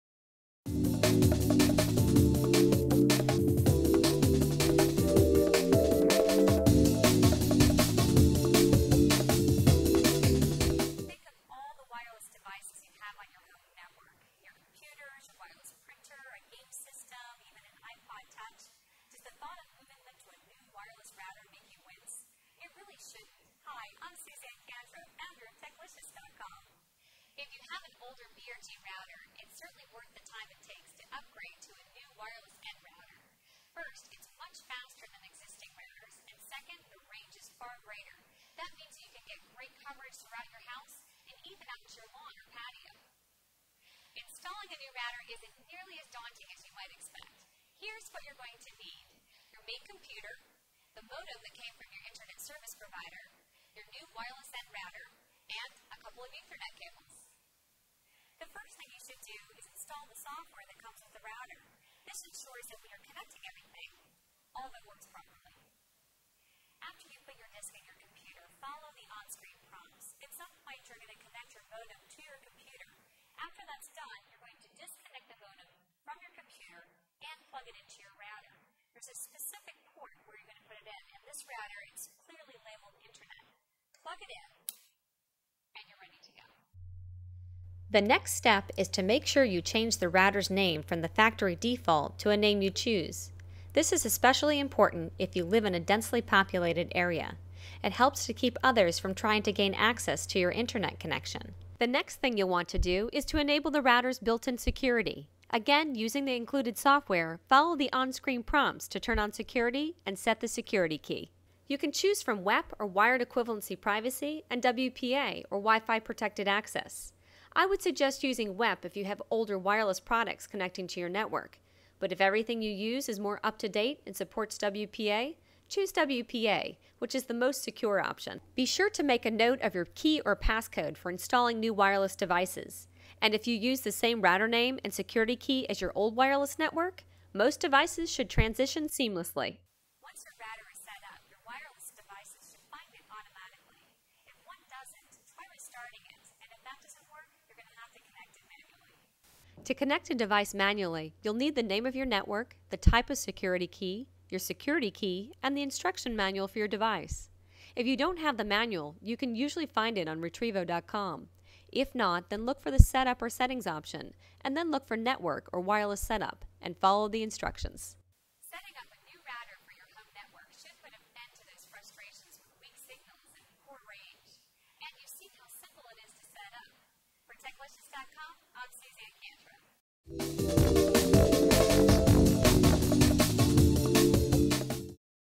How to Upgrade Your Wireless Network with a New Router
Increase the speed and range of your network by upgrading to a wireless 802.11n router.  These simple steps from Suzanne Kantra, Editor-in-Chief of Techlicious.com, show you how.